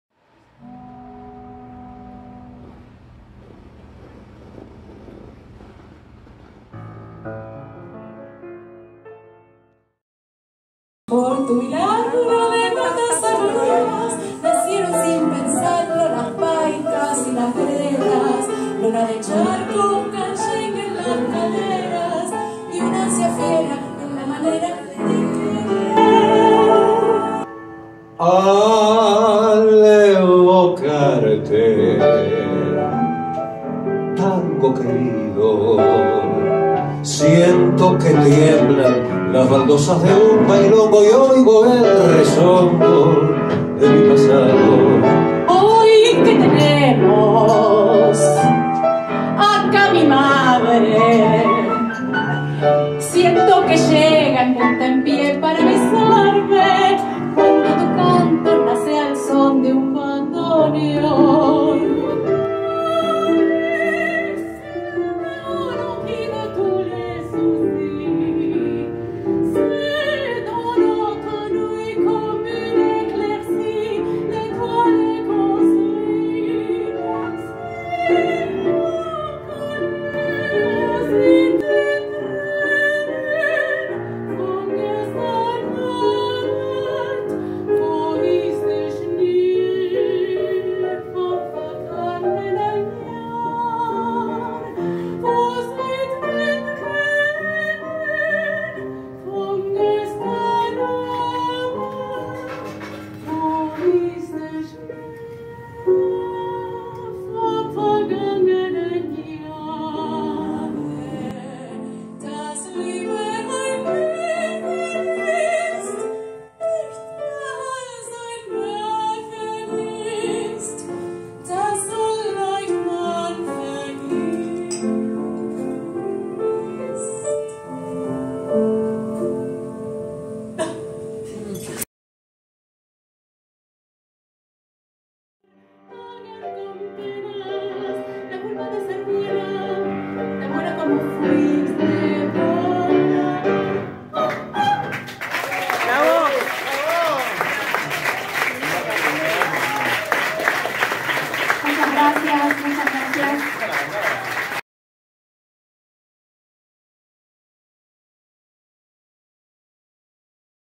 Tango Fieber: Underworld Songs - Trailer
Principios del Siglo XX. La Metrópolis, todos los caminos que conducen al tango se cruzan allí. Esto ha sido descifrado -también- por compositores de orígenes lejanos al Río de la Plata, geográfica y musicalmente.
Una invitación a navegar por las orillas del tango en diferentes continentes y épocas, junto a Eva Encanto (aka Natalia Hurst) y el Maestro Mariano Manzanelli.